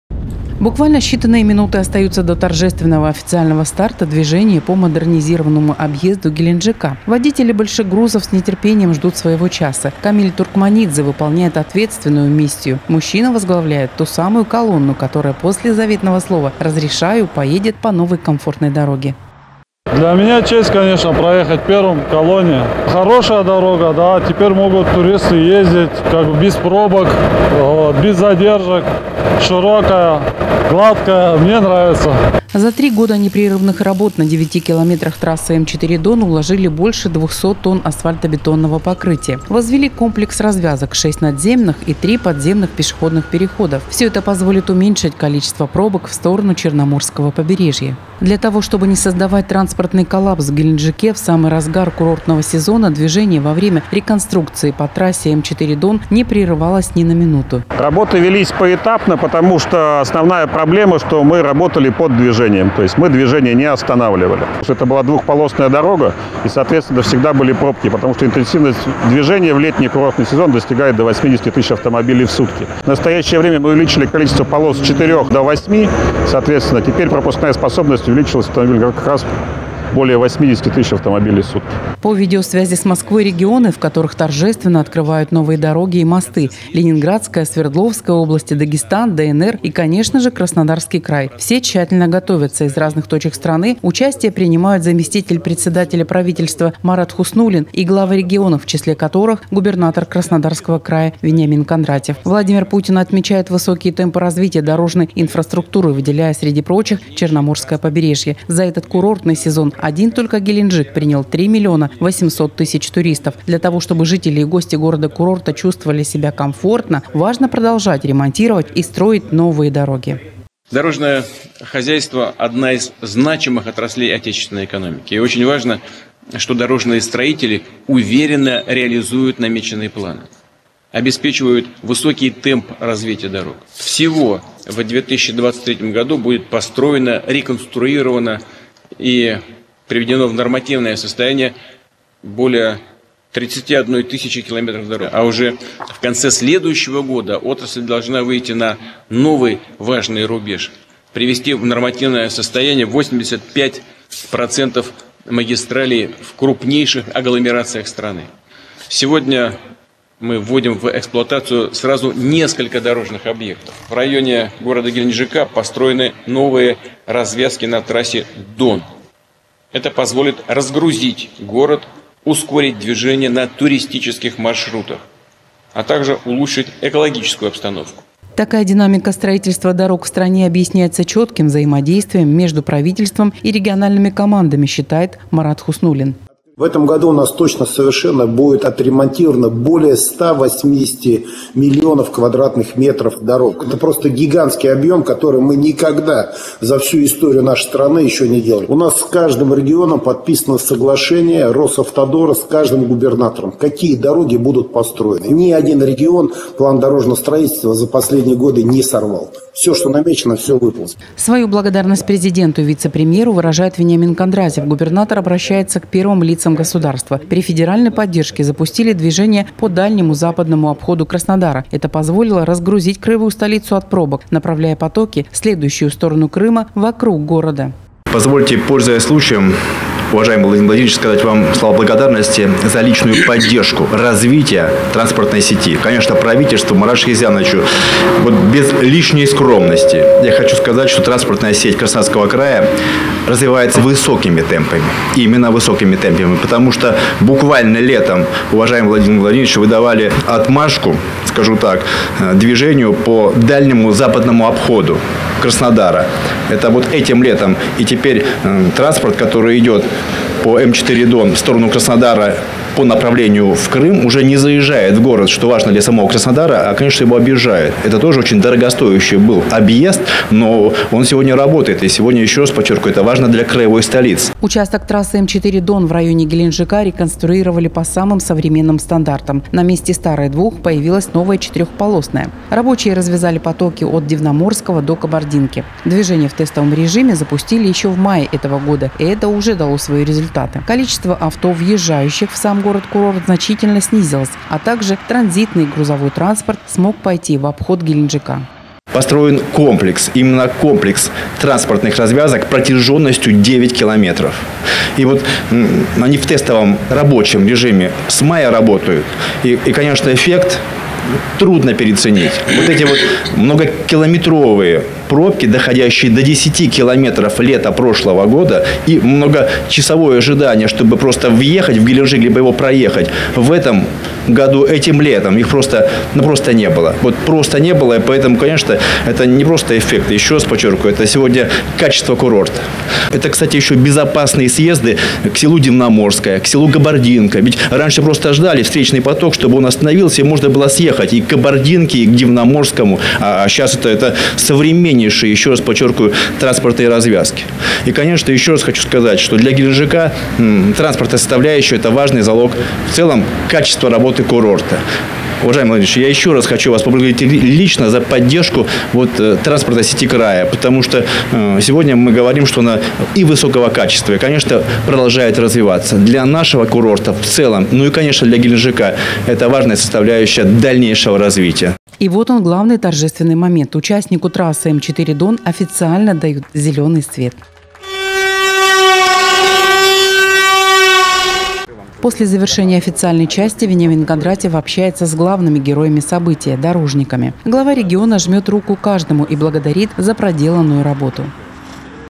Владимир Путин открыл проезд по реконструированному обходу Геленджика
Торжественная церемония состоялась по видеоконференцсвязи. В селекторе приняли участие зампредседателя Правительства Марат Хуснуллин, губернатор Краснодарского края Вениамин Кондратьев.
За 3 года непрерывных работ на 9 километрах трассы м4 дон уложили больше 200 тонн асфальтобетонного покрытия. Возвели комплекс развязок, 6 надземных и 3 подземных пешеходных переходов. Все это позволит уменьшить количество пробок в сторону черноморского побережья.
Для того, чтобы не создавать транспортный коллапс в Геленджике в самый разгар курортного сезона движение во время реконструкции по трассе м 4 дон не перерывалось ни на минуту.
По видеосвязи с Москвой - регионы, в которых торжественно открывают новые дороги и мосты: Ленинградская, Свердловская области, Дагестан, ДНР и конечно же Краснодарский край. Владимир Путин отмечает высокие темпы развития дорожной инфраструктуры, выделяя среди прочих черноморское побережье. За этот курортный сезон один только Геленджик принял 3 миллиона 800 тысяч туристов. Для того, чтобы жители и гости города-курорта чувствовали себя комфортно, важно продолжать ремонтировать и строить новые дороги.
Такая динамика строительства дорог в стране объясняется четким взаимодействием между правительством и региональными командами.
Свою благодарность президенту и вице-премьеру выражает Вениамин Кондратьев. Губернатор обращается к первым лицам государства: при федеральной поддержке запустили движение по дальнему западному обходу Краснодара. Это позволило разгрузить краевую столицу от пробок, направляя потоки, следующие в сторону Крыма, вокруг города.
Участок трассы М4 Дон в районе Геленджика реконструировали по самым современным стандартам. На месте старой двух - появилась новая четырёхполосная. Рабочие развязали потоки от дивноморского до Кабардинки. Движение в тестовом режиме запустили ещё в мае этого года, и это уже дало свои результаты. Количество авто, въезжающих в сам город-курорт, значительно снизилось, а также транзитный и грузовой транспорт смог пойти в обход Геленджика.